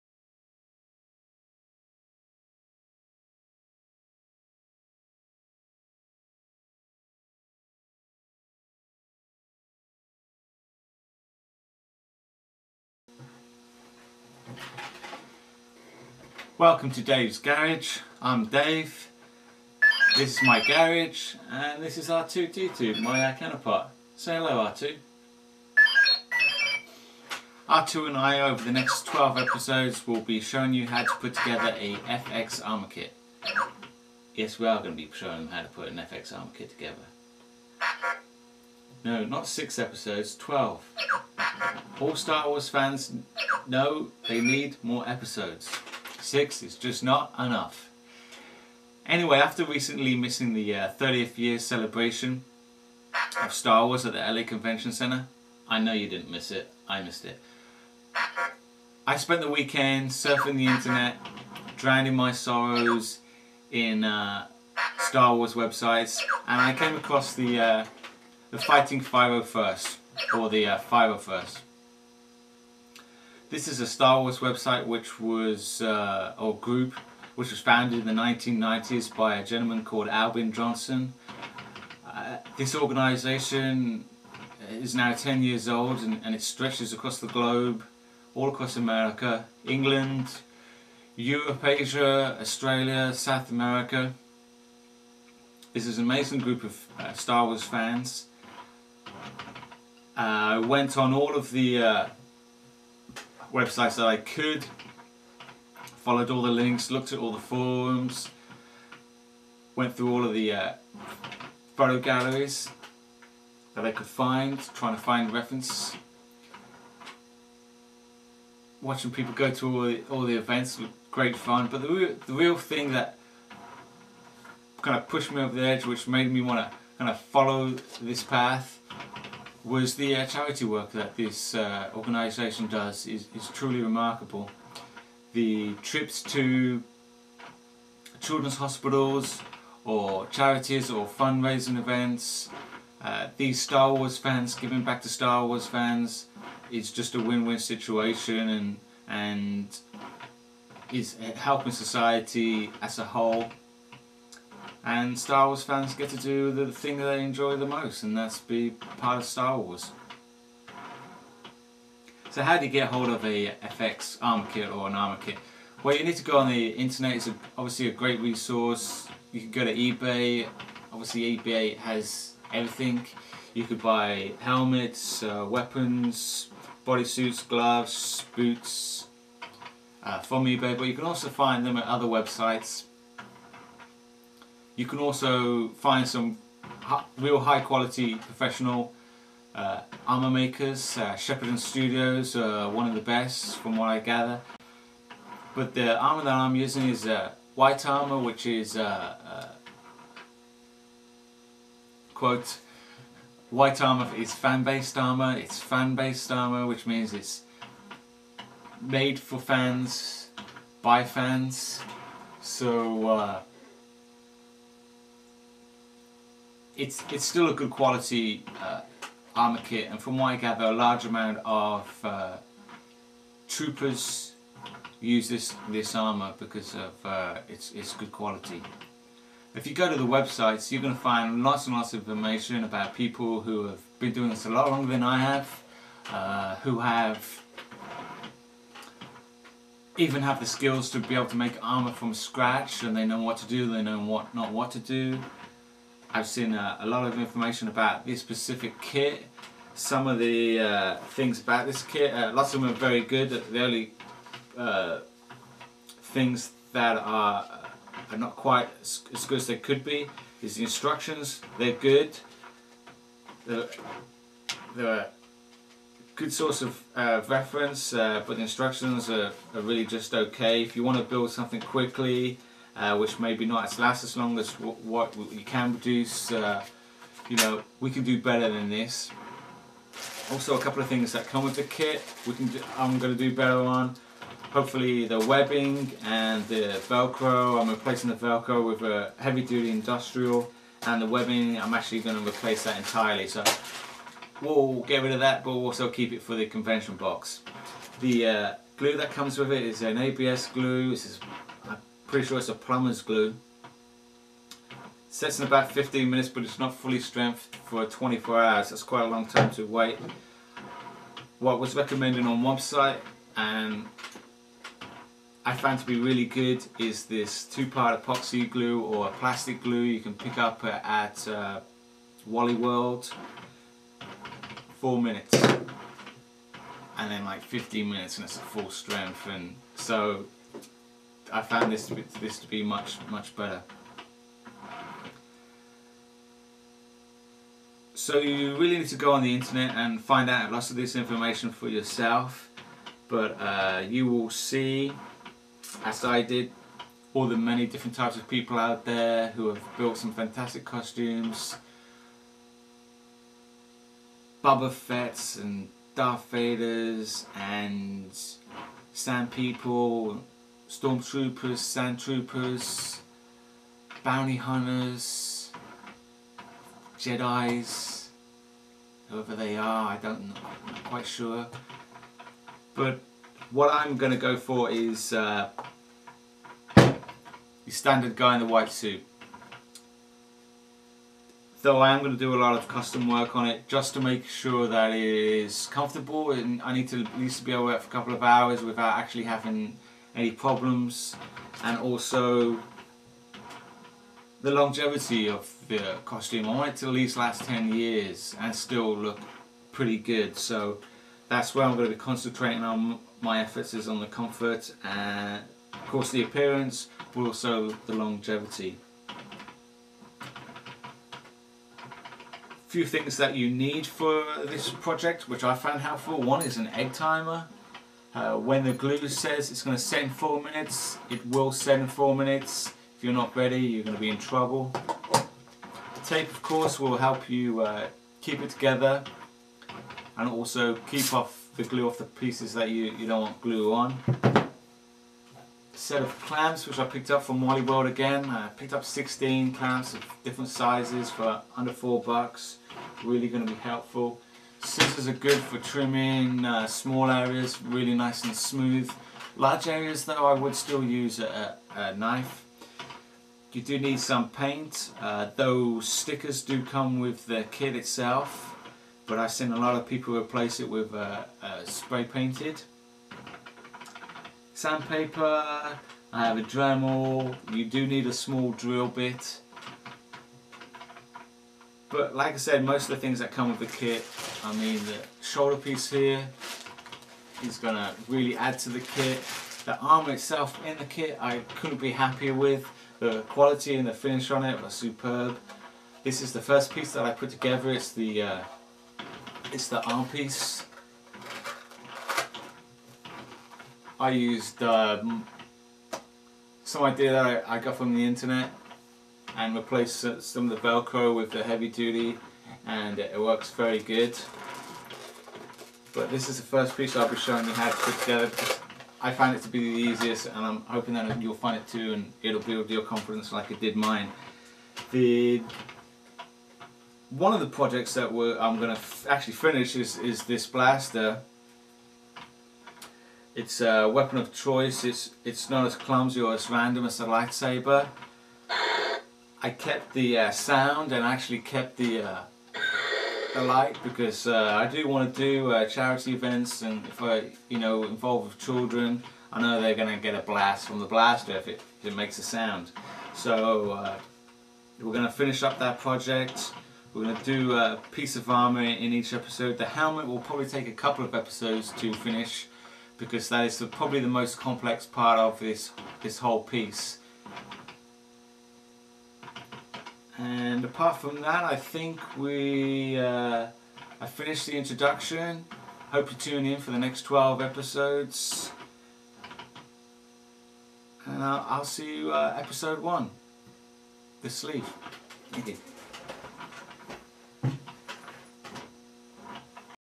Dave's Garage Introduction
Star Wars " White Armor " Project
A step by step process of assembling a FX armor kit with special guest R2!